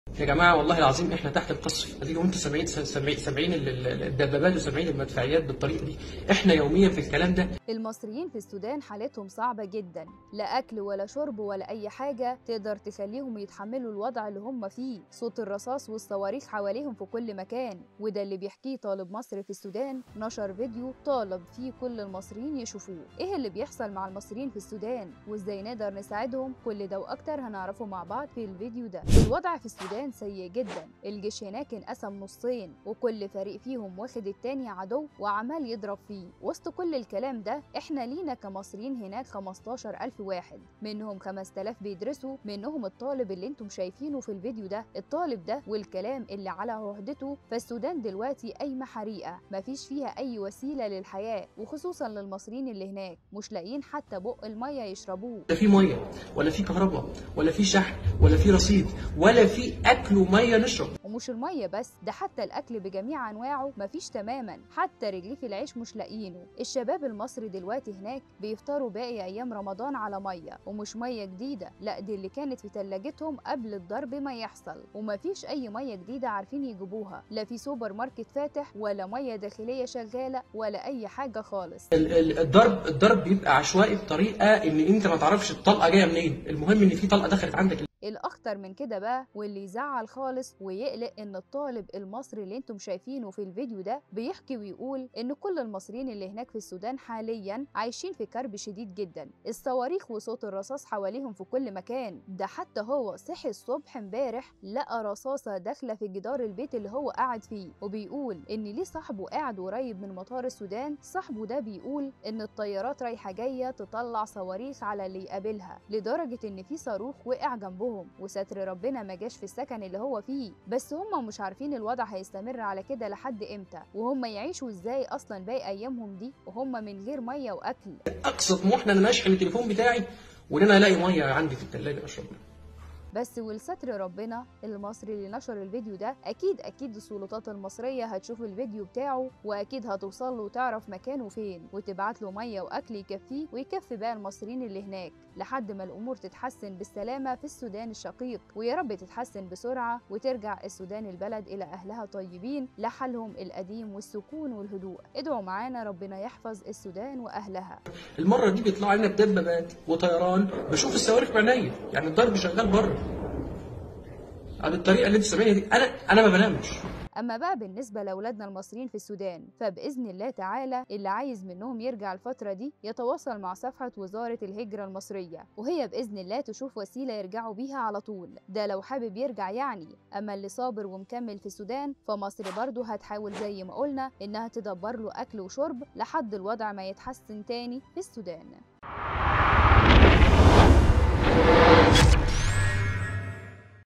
صوت الصواريخ في كل مكان.. طالب مصري في السودان يستغيث من اللي بيحصل معاهم: مش هتصدق عايشين إزاي؟!
بس ولستر ربنا المصري اللى نشر الفيديو ده أكيد أكيد السلطات المصرية هتشوف الفيديو بتاعه واكيد هتوصله وتعرف مكانه فين وتبعتله مايه وأكل يكفيه ويكفى باقى المصريين اللى هناك لحد ما الأمور تتحسن بالسلامه فى السودان الشقيق، ويارب تتحسن بسرعه وترجع السودان البلد اللى أهلها طيبين لحالها القديم والسكون والهدوء، أدعوا معانا ربنا يحفظ السودان وأهلها